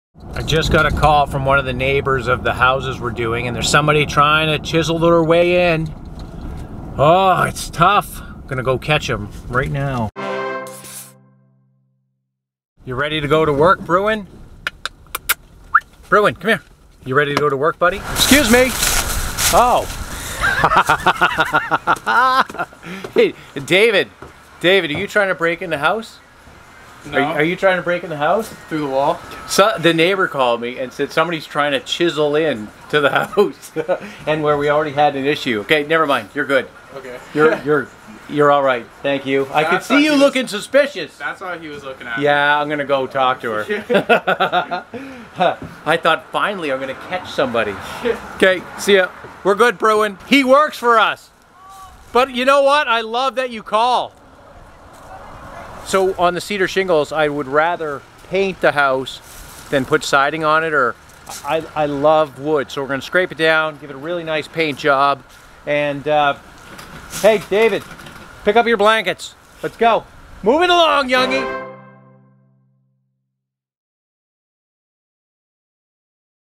You Look Suspicious!
Day sixteen!  Rob gets a call from a concerned neighbour that somebody is trying to break into a property on Bromley Ave.